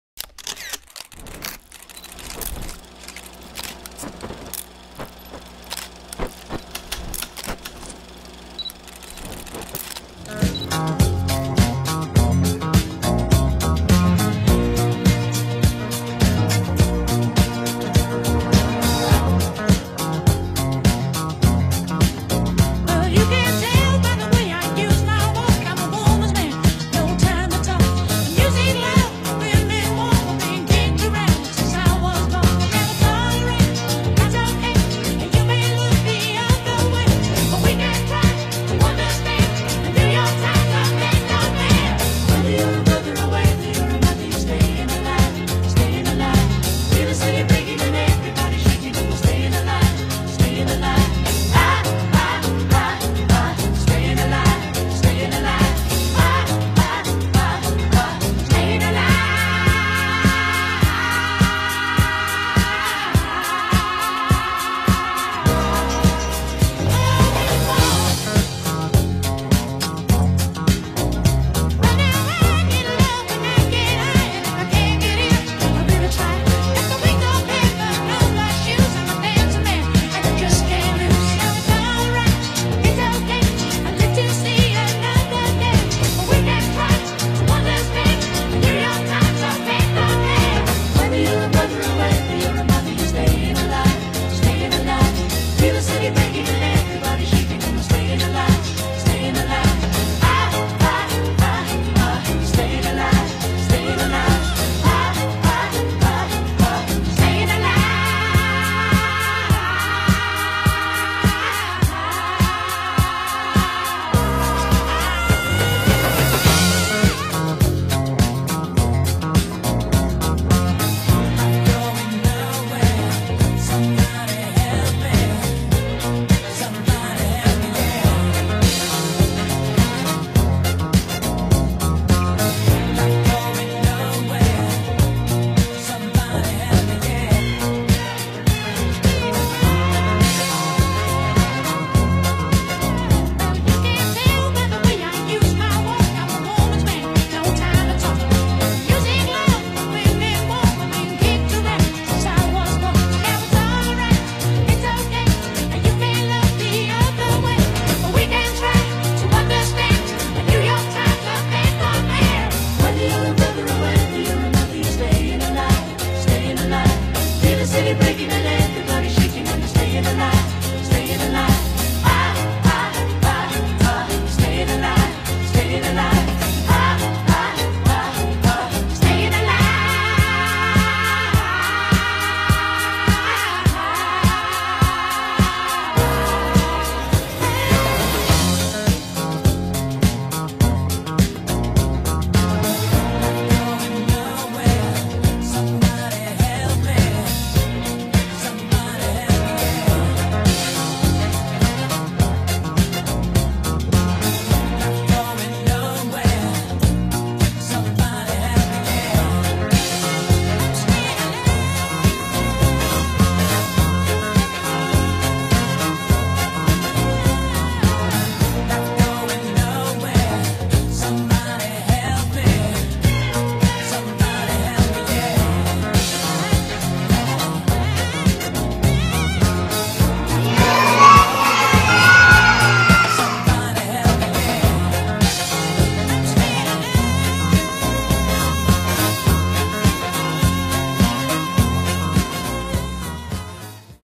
Music & Dance 🪩 (Summer Fun 2024)
🕺🎶 Step back in time with us as we groove through the decades! From the 1960s to the 1990s, our first week of SummerFun was filled with music, dance, crafts, games, and water play. Both students and teachers had a blast, making memories and enjoying every moment of our retro journey! 💃🏻🪩